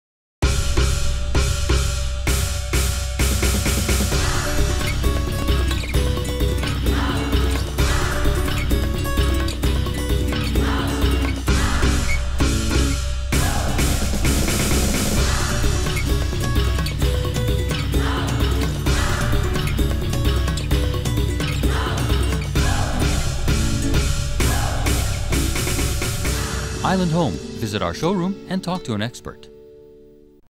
Island_Home Final.mov
Island Home Furniture Commercial
Composed and Produced by Danny McIntyre and Joey Vierra at Blue Drip Studios, Honolulu, Hawaii.